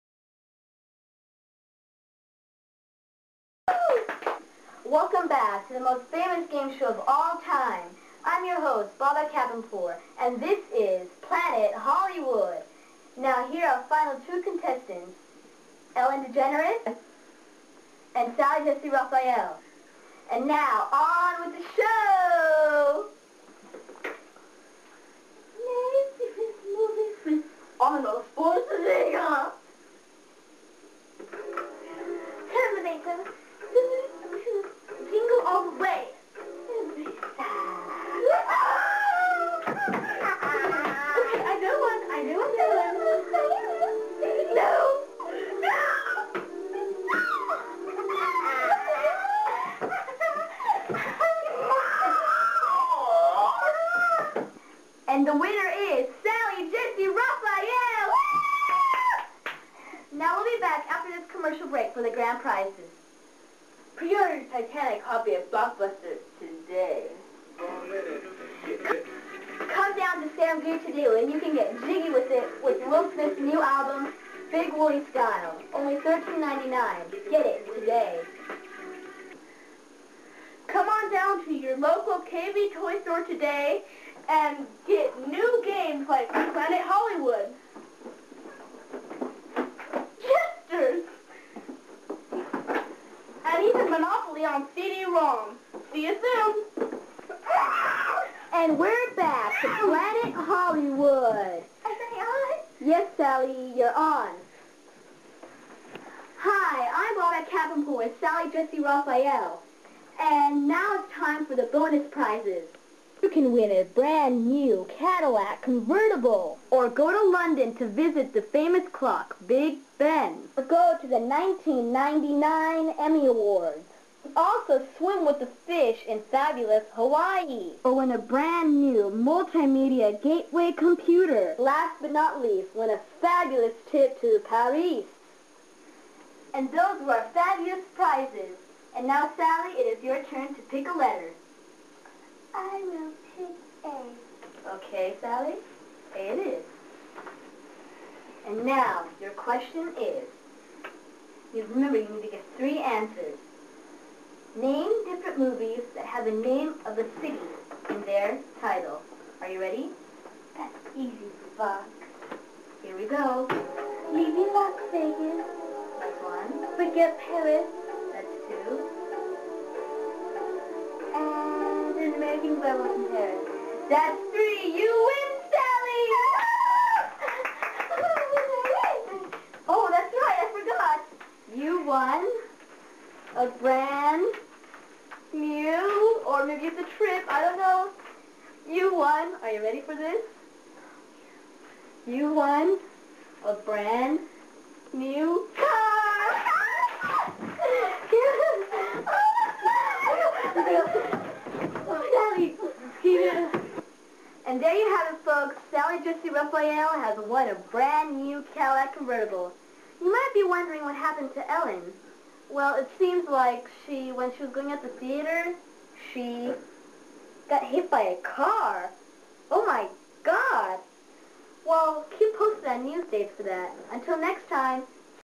Planet Hollywood
Sally Jesse Raphael and Ellen Degenerous compete on Planet Hollywood